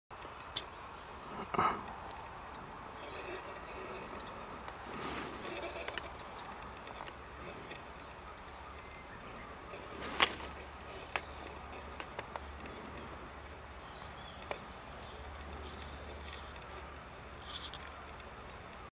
キマダラルリツバメ Cigaritis takanonis / Spindasis takanonis 福島県三島町 2015/07/06 butterfly of Japan
A Wild Butterfly Chase　蝶をめぐる冒険
butterfly of Japan　日本の蝶